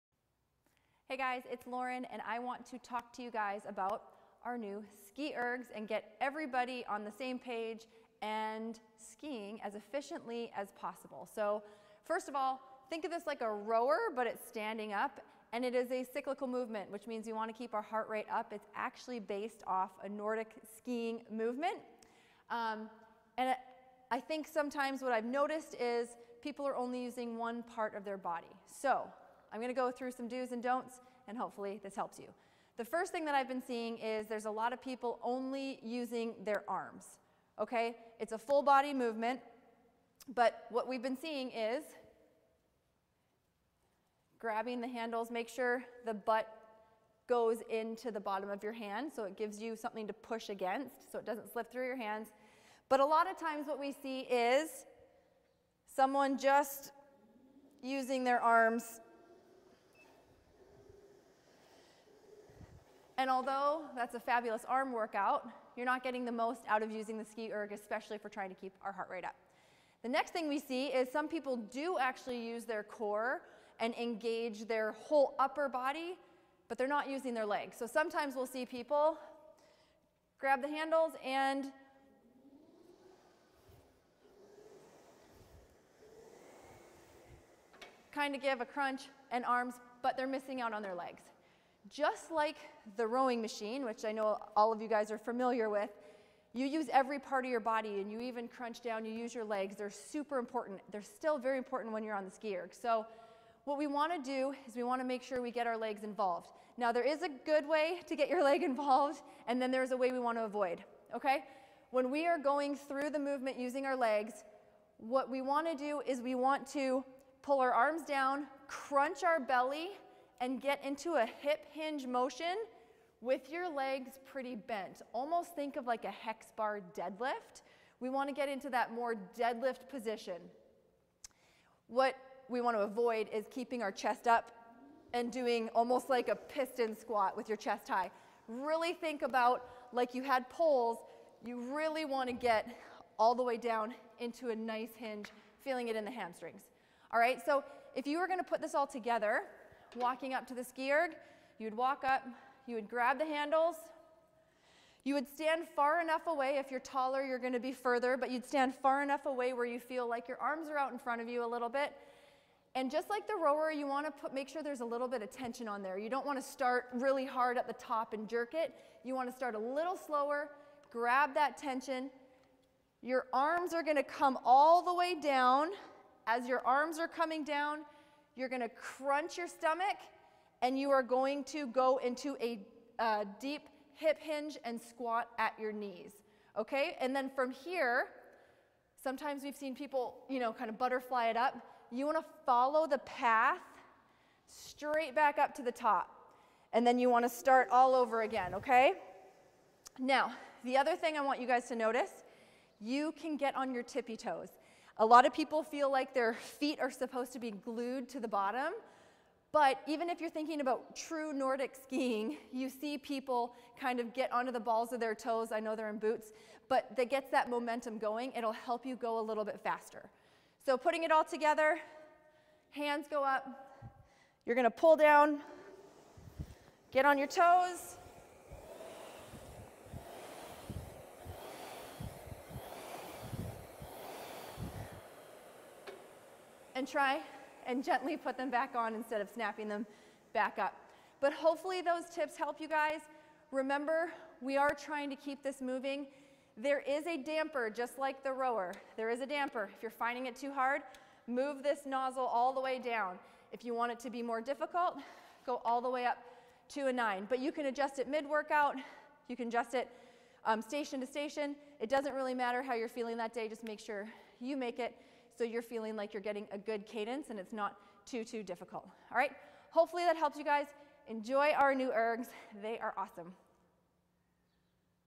Ski Erg 101 - How to Use a Ski Erg (TrAK Athletics)
5 Tips from a 2x Olympian on How to Use the Ski Erg
Lauren Regula, 2x Olympian, 3x Mom, and owner of TrAk Athletics breaks down the dos and don'ts of this amazing conditioning machine.